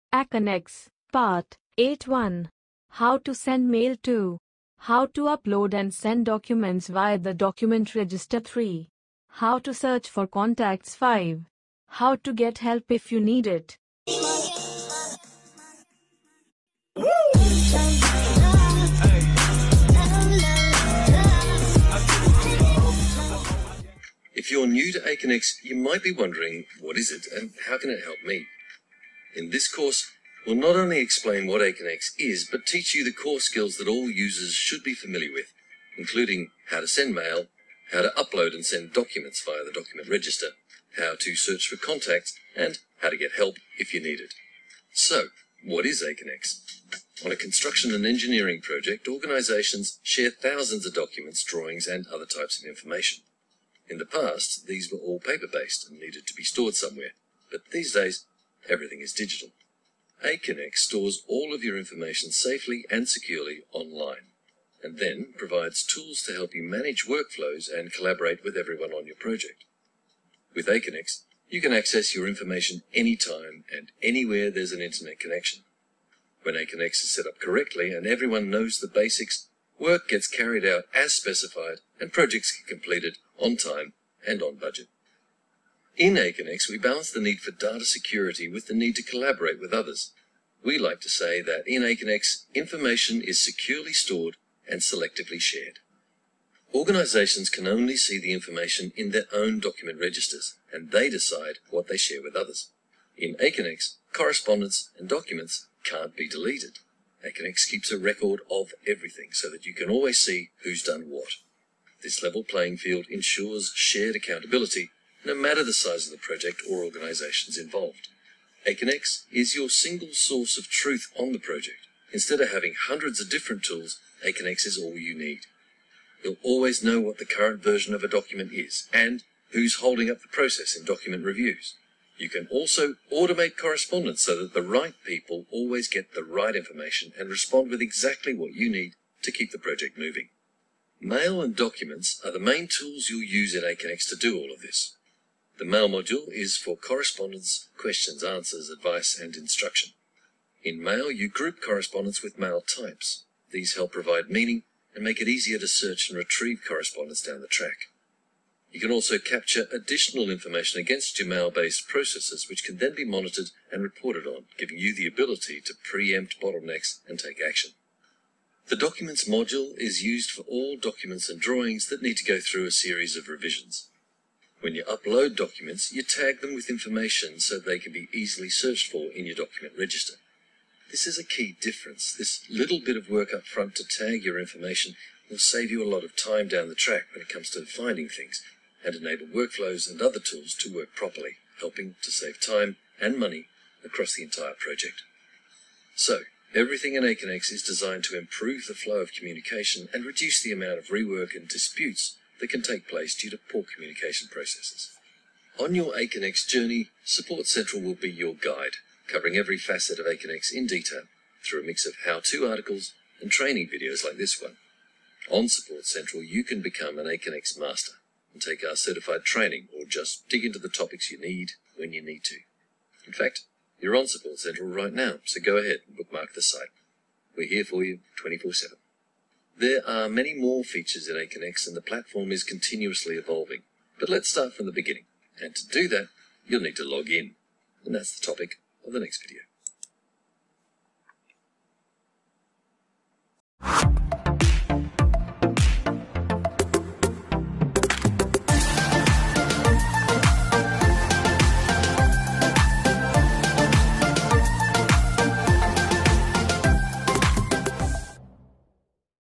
Aconex I Part - 8 I Send Mail I Upload via Doc Register I Search Doc. I How to get Help
This video shows step by step on aconex as a document controller. aconex as an edms helps working on the documents stress free.